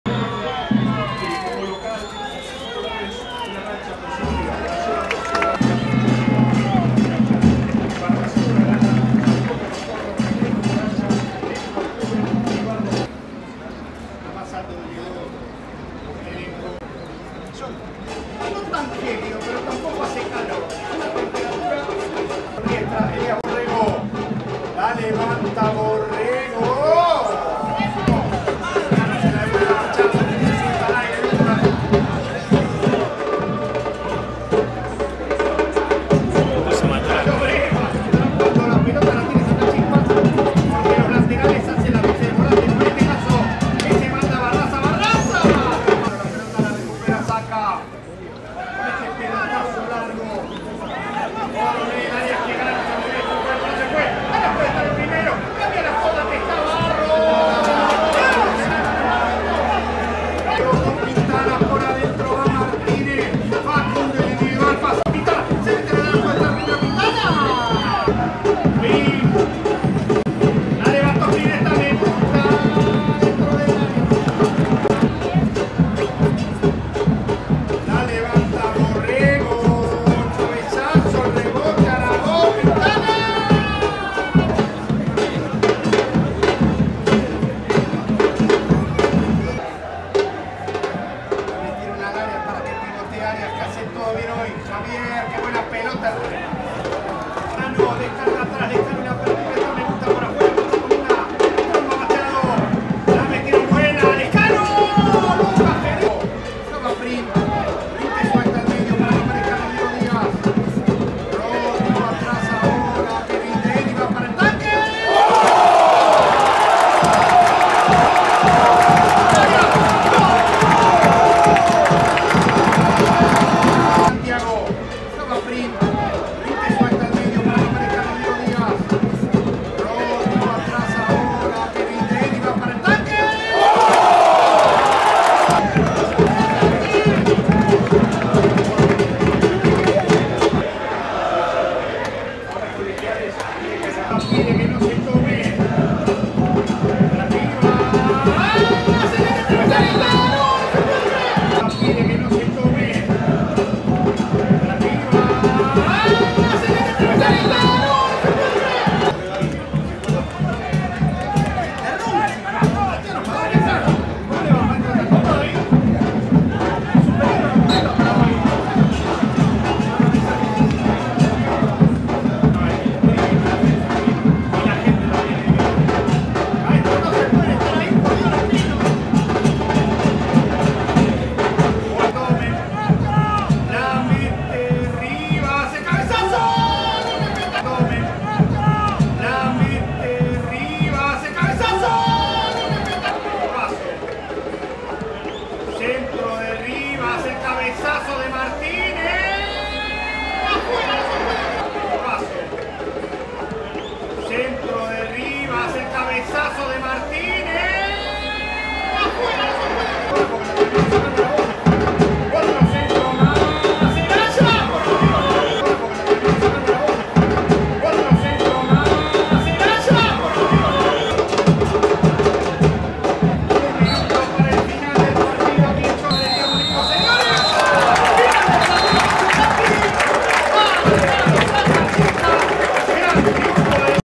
Sacachispas 1-0 Colegiales | Primera División B | Fecha 16 (Apertura)
Gol de Héctor Javier Arias a los 72'.

Seguí toda la Primera B en TyC Sports y TyC Sports Play.